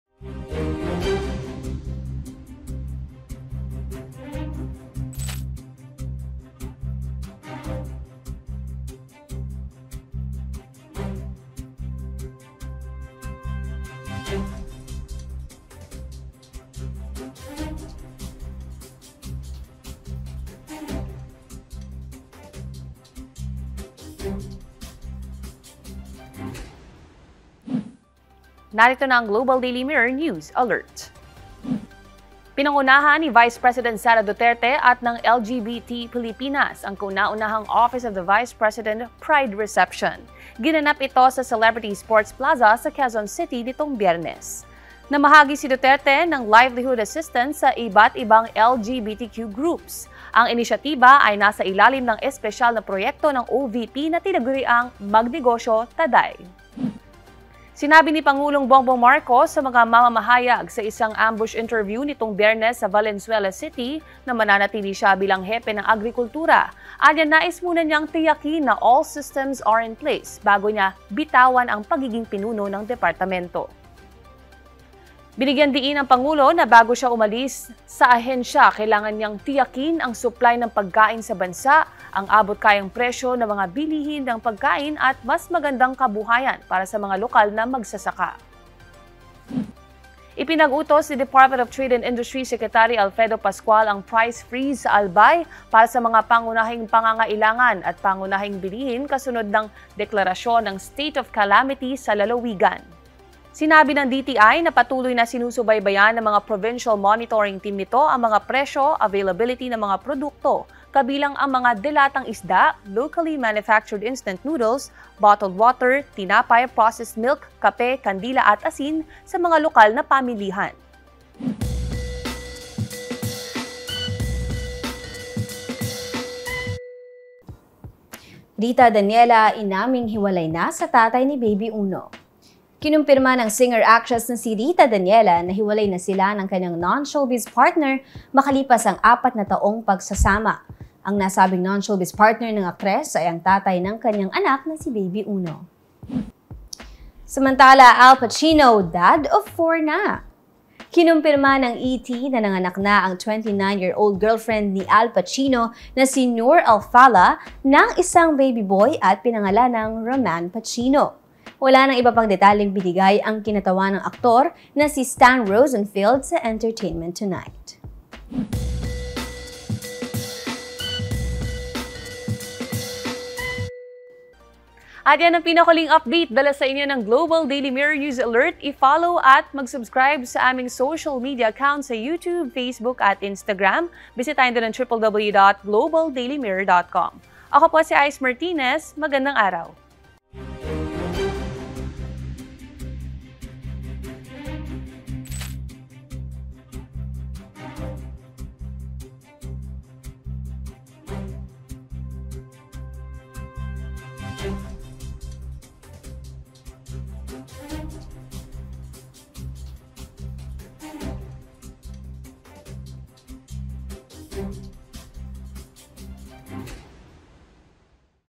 GDM News Alert - Biyernes, Hunyo 16, 2023
GDMNewsAlert| Ikaw ay nanonood ng Global Daily Mirror News Alert. Narito ang mga headlines ngayong Hunyo 16, ibabalita nila Ice Martinez and Leslie Ordinario

-OVP pinangunahan ang isang LGBTQIA+ party

-Marcos mananatili pa rin bilang DA Chief

-DTI iniutos ang price freeze ng mga basic goods sa Albay

-Rita Daniela, inaming hiwalay na sa tatay ni baby Uno

-Al Pacino dad of 4 na!